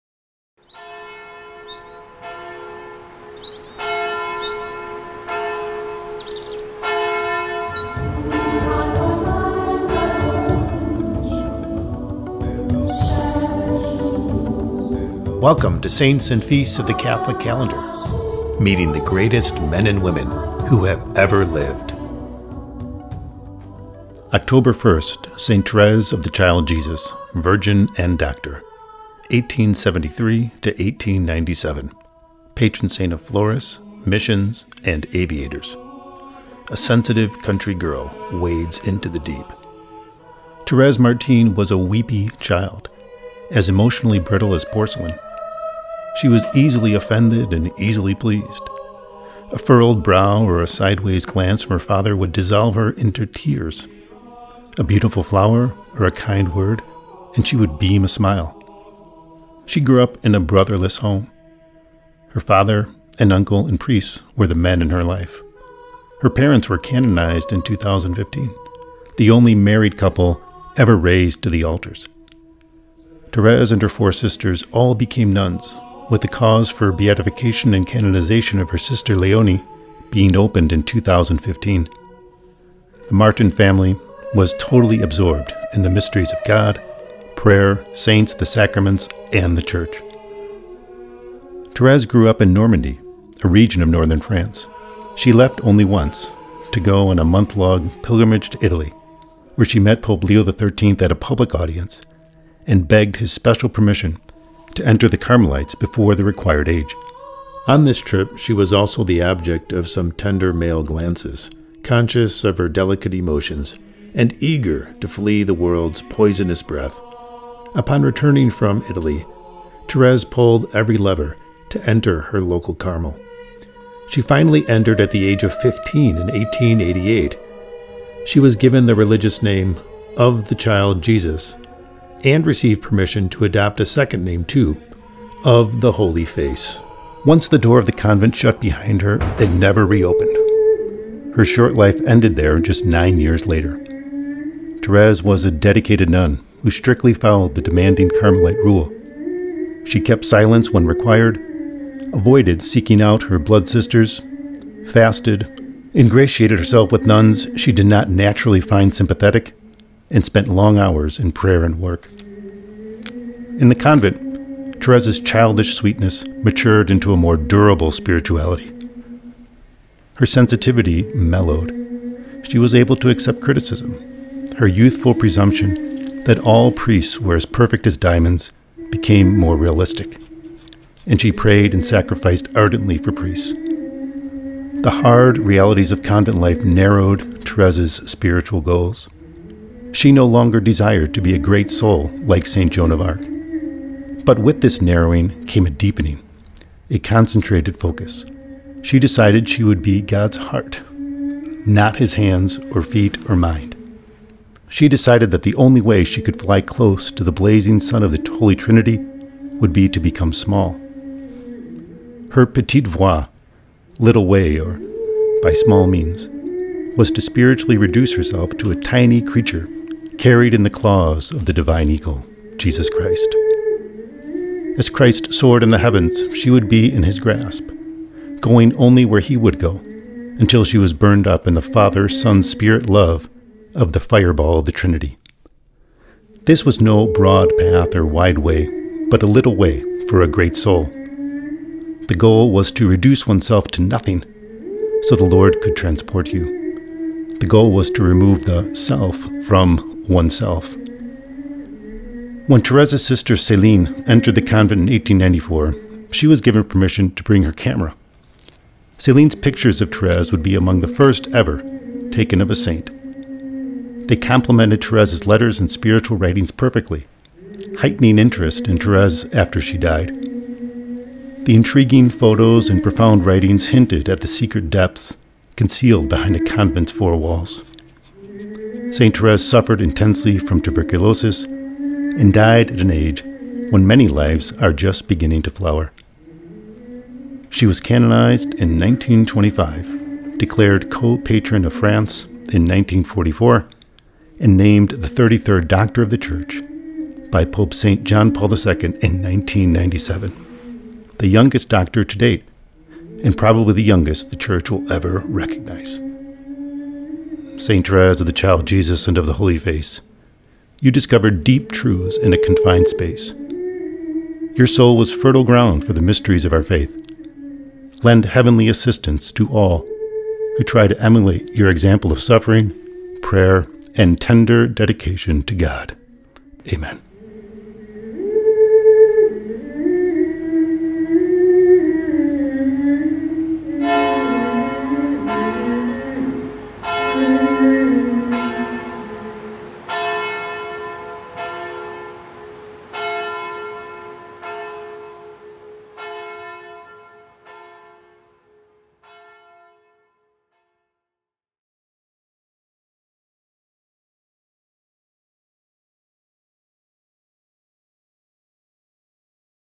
October 1: Saint Thérèse of the Child Jesus, Virgin & Doctor
A sensitive country girl wades into the deep

Thérèse Martin was a weepy child, as emotionally brittle as porcelain. She was easily offended and easily pleased. A furled brow or a sideways glance from her father would dissolve her into tears. A beautiful flower or a kind word and she would beam a smile. She grew up in a brotherless home. Her father, an uncle, and priests were the men in her life. Her parents were canonized in 2015, the only married couple ever raised to the altars. Thérèse and her four sisters all became nuns, with the cause for beatification and canonization of her sister Léonie being opened in 2015. The Martin home was totally absorbed in the mysteries of God, prayer, saints, the Sacraments, and the Church.

Thérèse grew up in Normandy, a region of Northern France. She left only once, to go on a month-long pilgrimage to Italy, where she met Pope Leo XIII at a public audience and begged his special permission to enter the Carmelites before the required age. On this trip she was also the object of some tender male glances. Conscious of her delicate emotions and eager to flee the world’s...